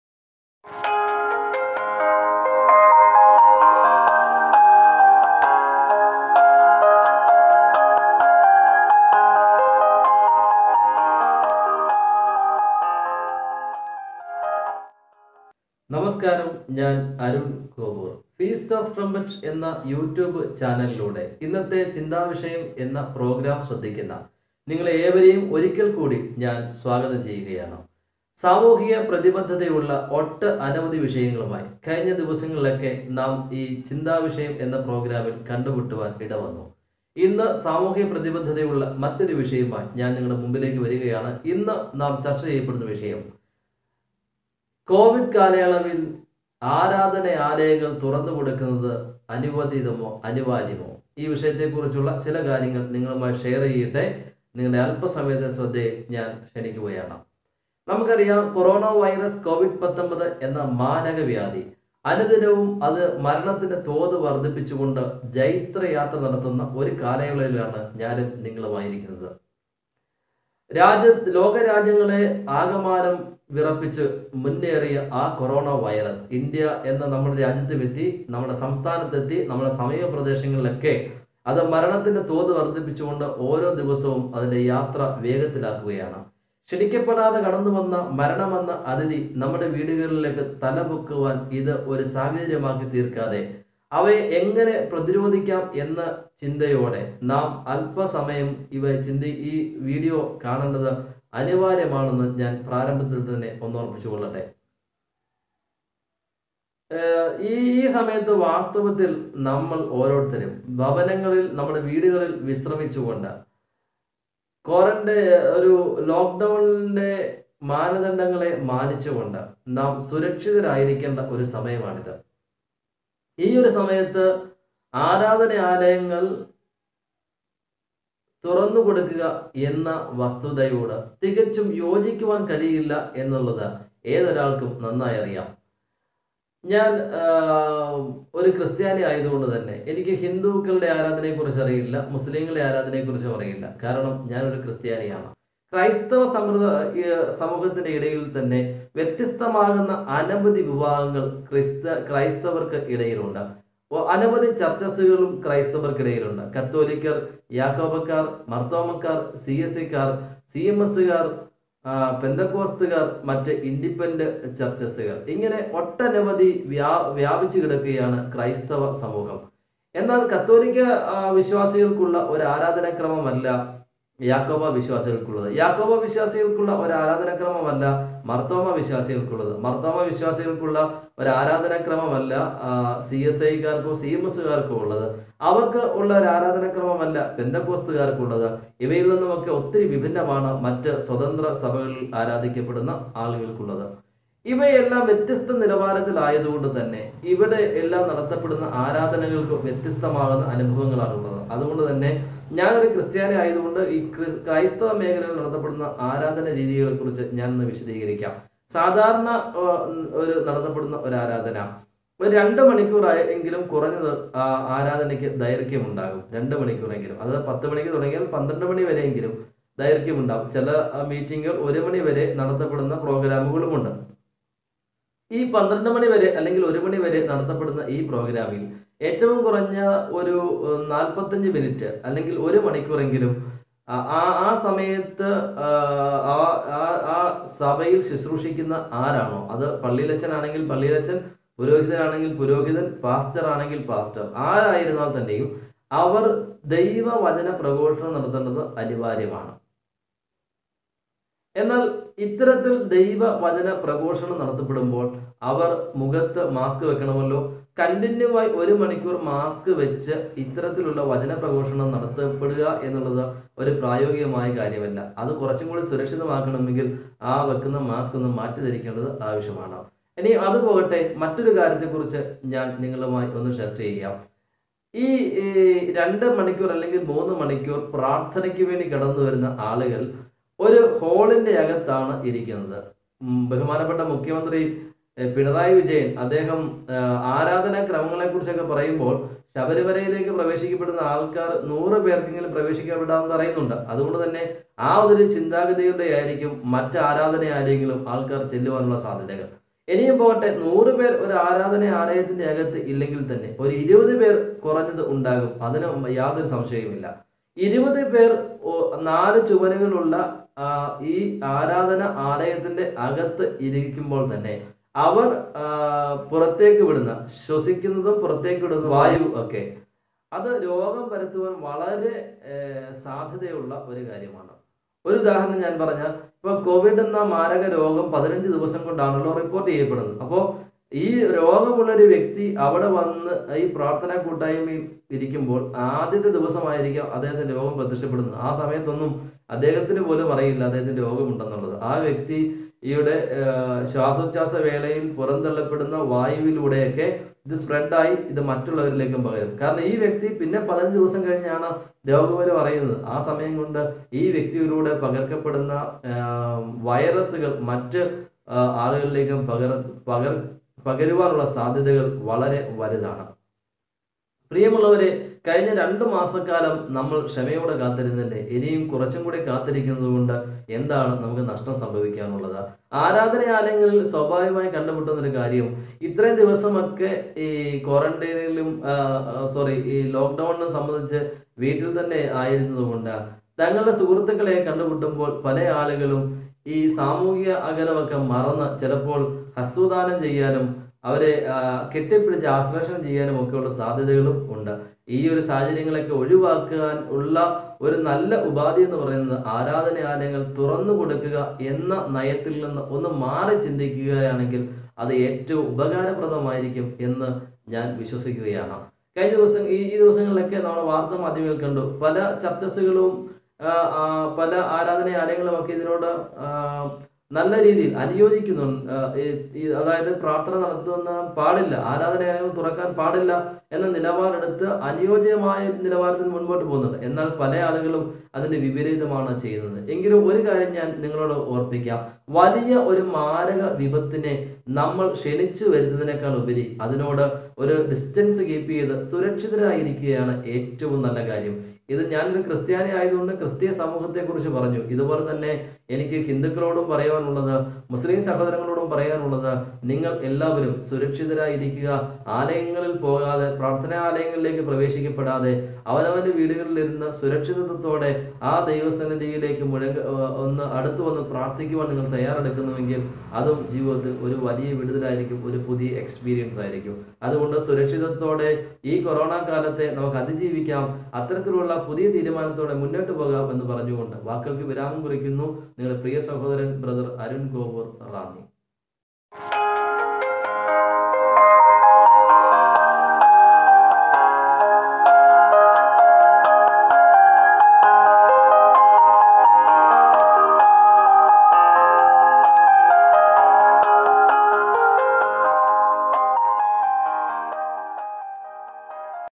കോവിഡ് കാലയളവിൽ ആരാധനാലയങ്ങൾ തുറന്ന് കൊടുക്കുന്നത് അനുവാര്യമോ??....
ഇന്നത്തെ ചിന്താവിഷയം
Br . Arun kovoor